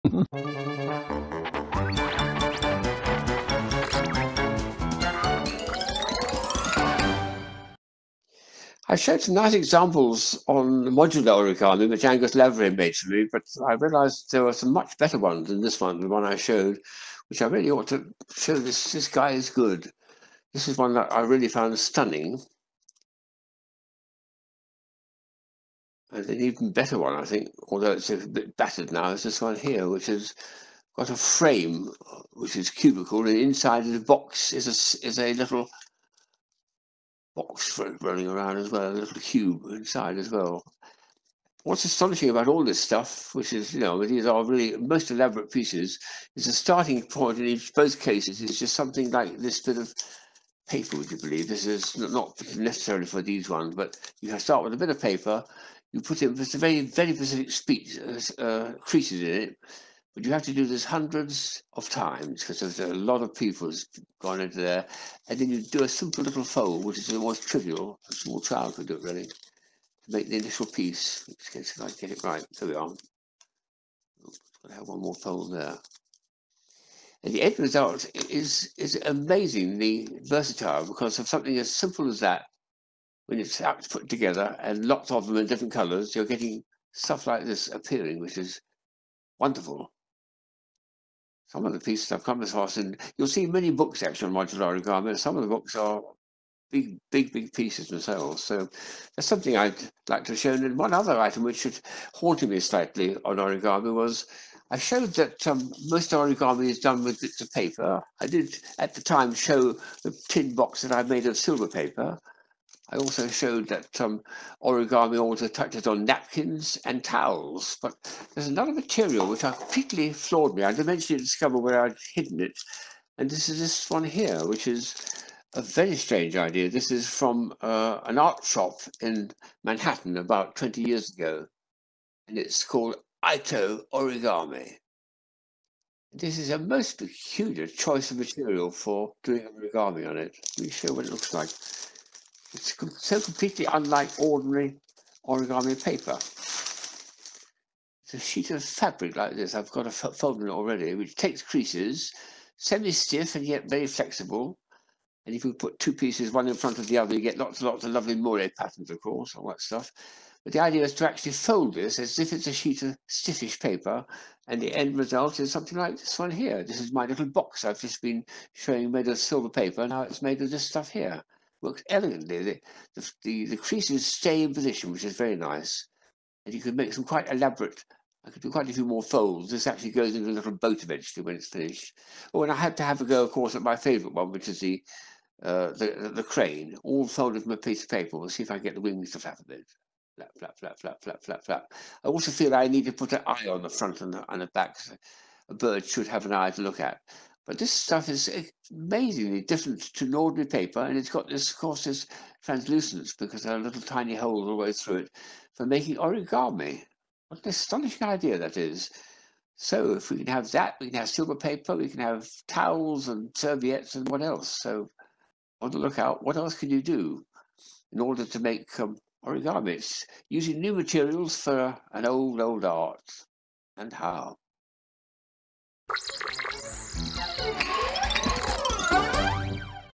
Tim's Unusual Origami
In an earlier video, Tim showed some examples of 'modular origami' - where you use multiple sheets of origami paper, often sheets of different colours, to create the finished product, rather than the traditional method of using a single sheet. Tim showed some examples he had got from Angus Lavery, who is an expert at origami. However Tim was sure he had some other examples, even more amazing ones, made by Angus,  and now he has managed to find them.

The first example shows 4 cubes, made with a red and white design, with a linking structure in blue. It almost looks like some kind of space ship! The second one shows a multi colour cube, trapped within a cubic framework.

Tim has mentioned previously that as well as paper, you can fold napkins and towels, even silver paper. But there was another really unusual material he remembered, and now he has found it! Bought from an art shop in Manhattan about 20 years ago, it is called Aitoh Origami. Also called ColorMesh Origami, it says on the packet that it is 100% rayon mesh, and it was made in Japan.

It is like a sheet of fabric, but it takes creases, and you can use it to make origami shapes. Tim shows his box design, made from this new material, and also the famous crane. So, as Tim says, a new material for an old, old art!